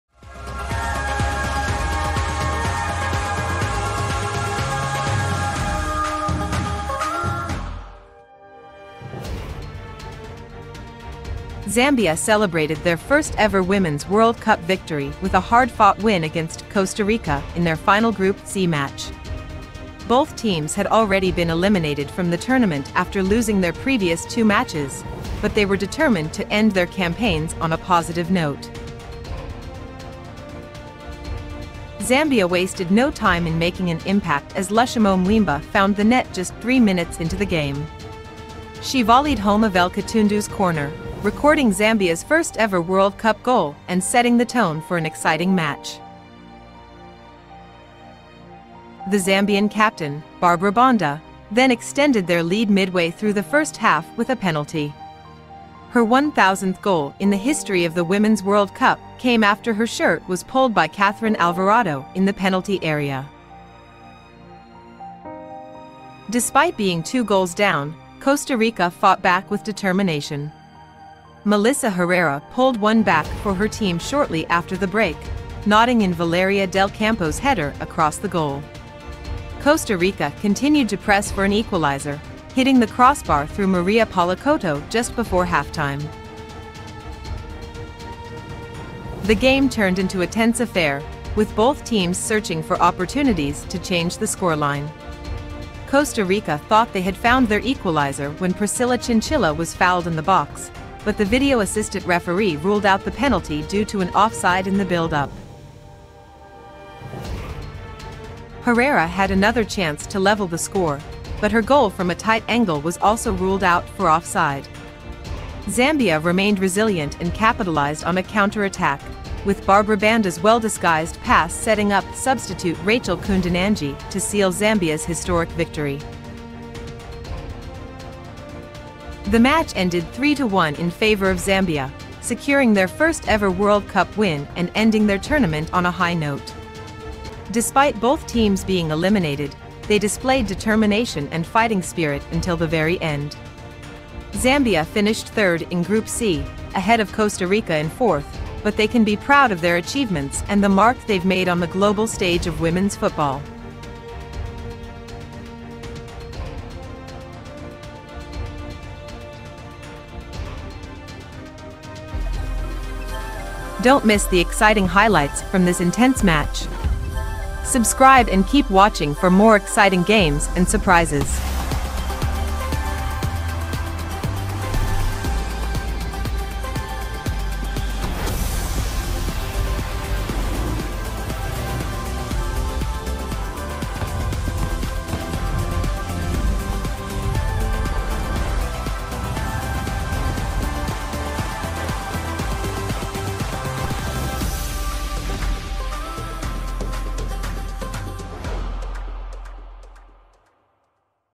Costa Rica vs Zambia | Group C | FIFA Women's World Cup 2023 | Highlights
Experience Zambia's historic Women's World Cup triumph as they secured their first-ever win by defeating Costa Rica in a thrilling Group C clash. Lushomo Mweemba's early goal set the tone, followed by Barbra Banda's penalty. Though Costa Rica pulled one back, Racheal Kundananji's late strike sealed Zambia's victory. Both teams had already been eliminated from the tournament, but Zambia's spirited performance ended their campaign on a high note. Witness the excitement and drama of this game in the FIFA Women's World Cup 2023 Highlights. Join us for more incredible moments from the world's premier women's football event.